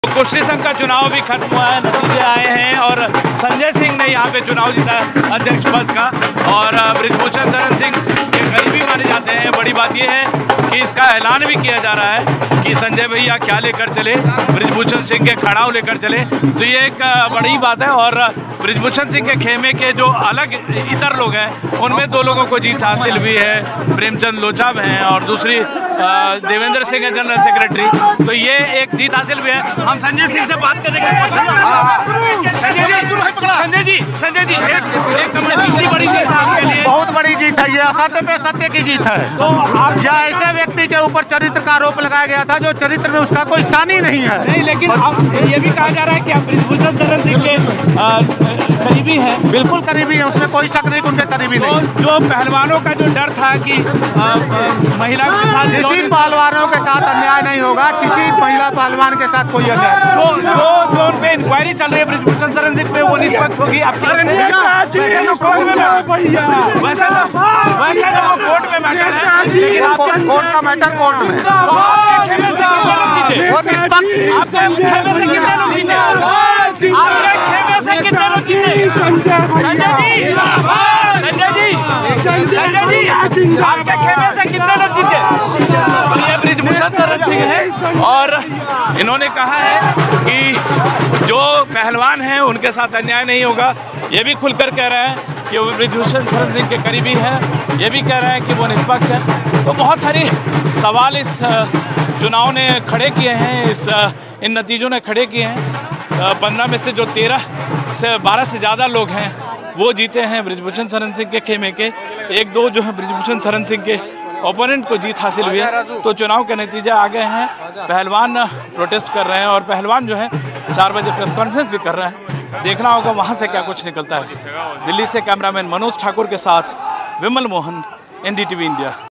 Brij Bhushan Aide On Wrestling Body Polls: "Victory Of Truth Over Lies"
In his first reaction after winning the controversial elections to the Wrestling Federation of India, Sanjay Singh has called it a big victory of truth over lies. Referring to BJP MP and former chief of the wrestling body, Brij Bhushan Sharan Singh, who faced sexual harassment allegations from top wrestlers, Sanjay Singh said an attempt was made to tarnish the reputation of a person whose character was spotless.